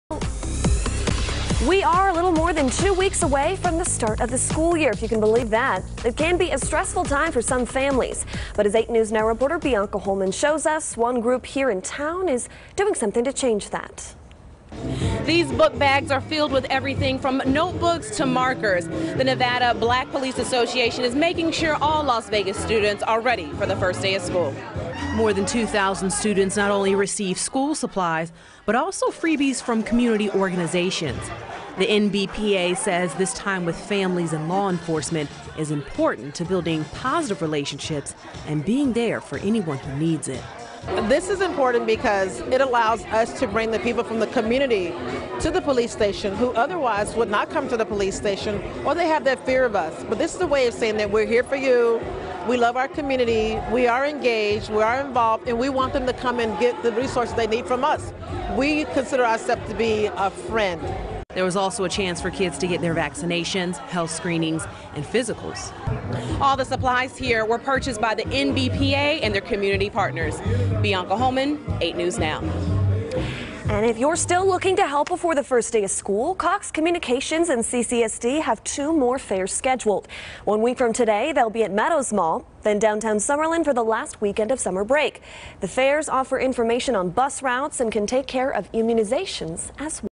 Nevada Black Police Association hosts Back to School fair
Today, the Nevada Black Police Association provided school supplies to Las Vegas valley students in need at the LVMPD Headquarters on S. Martin L. King Blvd.